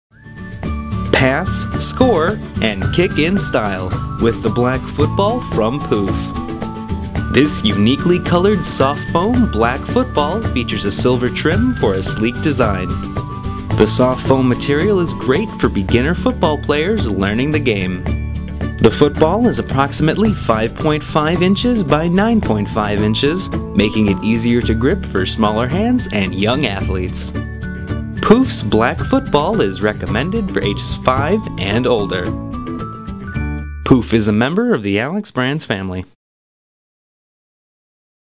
POOF Pro Gold Black Football 471TL
This black football is black with silver trim and is approximately 5.5″ x 9.5″